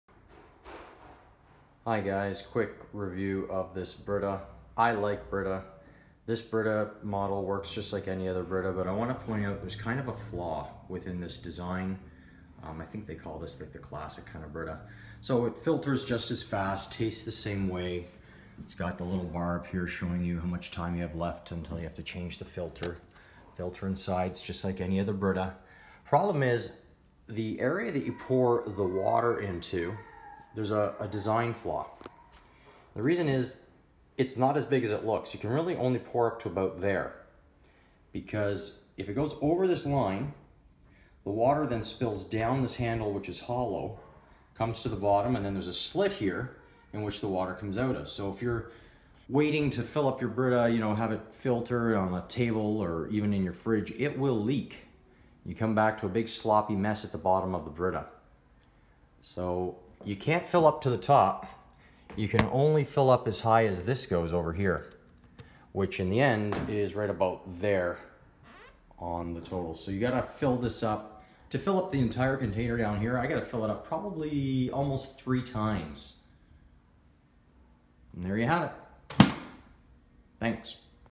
Brita Water Pitcher Review
THE GOOD:
-filters quickly
-water tastes pure
-durable/strong
-filter lasts about 90 days for a small family

THE BAD:
-design flaw that I mention in the video